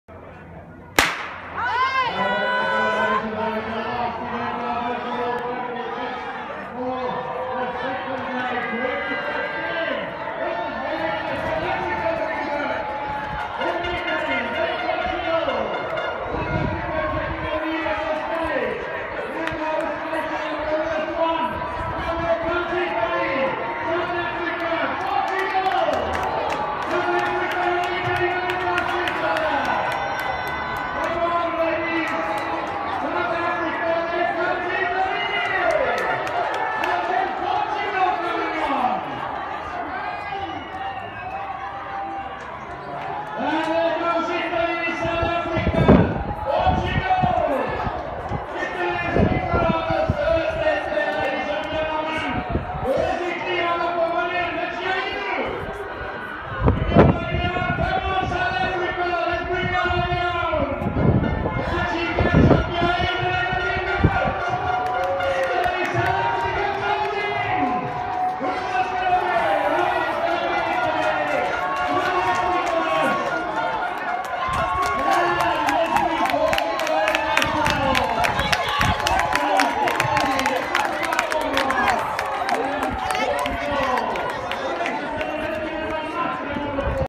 Mondiali IAADS 2015 Bloemfontein
Staffetta 4x100 femminile: ORO per l'Italia di Pertile, Bonfanti, Spano e Orlando